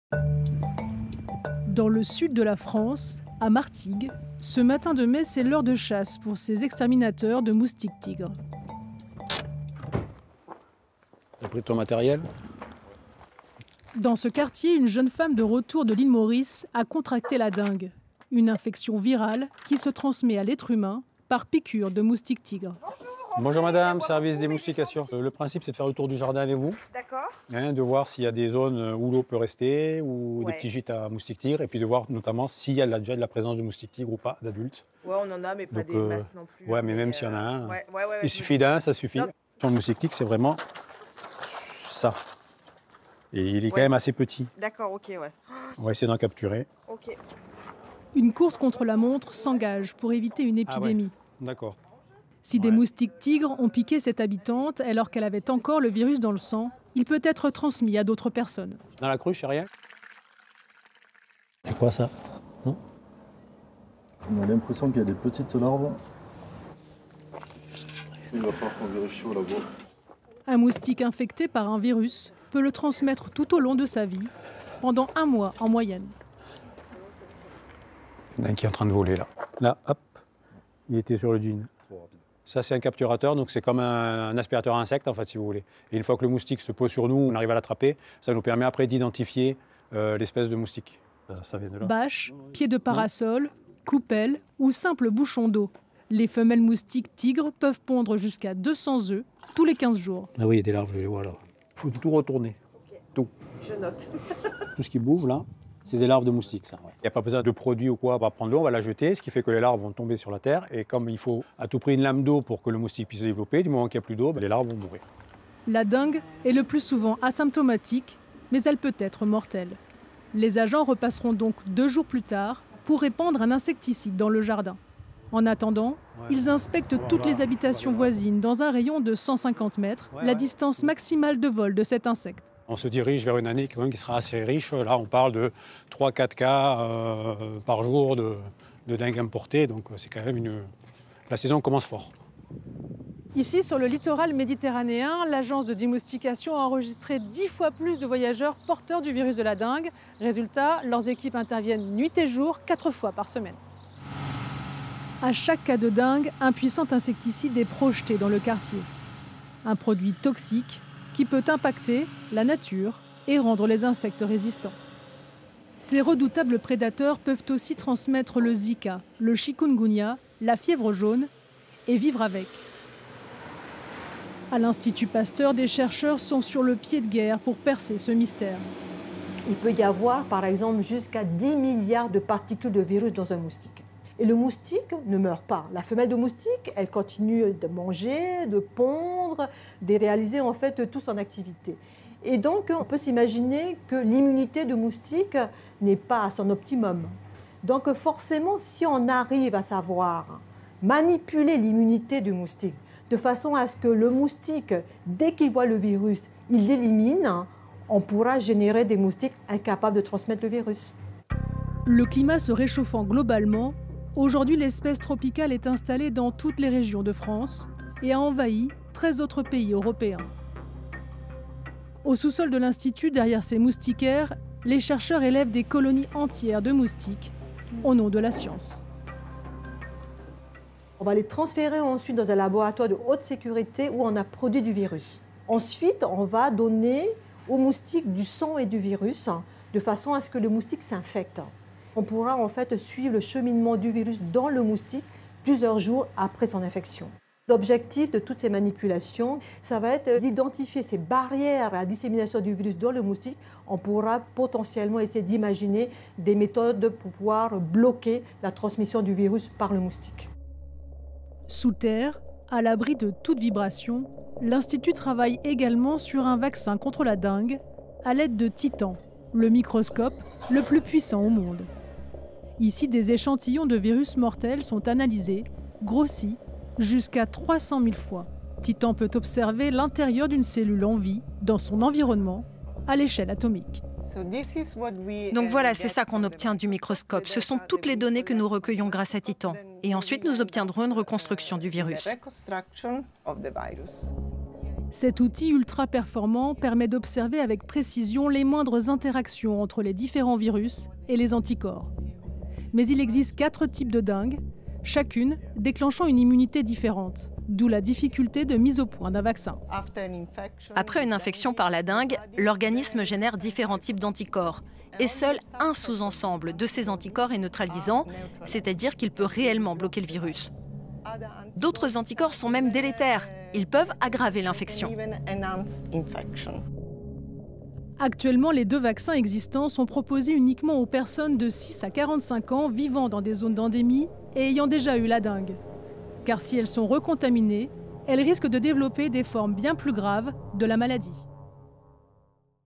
Record de cas de dengue en France • FRANCE 24
Plus de 2 000 cas de dengue importés ont été recensés en France métropolitaine depuis janvier, un record. C’est treize fois plus que l’an dernier pour la même période. L'Europe doit se préparer à une augmentation des infections transmises par le moustique-tigre. C'est la seule espèce dans l'Hexagone à transmettre plusieurs maladies potentiellement mortelles, comme la fièvre jaune, Zika, le chikungunya ou la dengue. L'insecte tigré et silencieux agit le jour.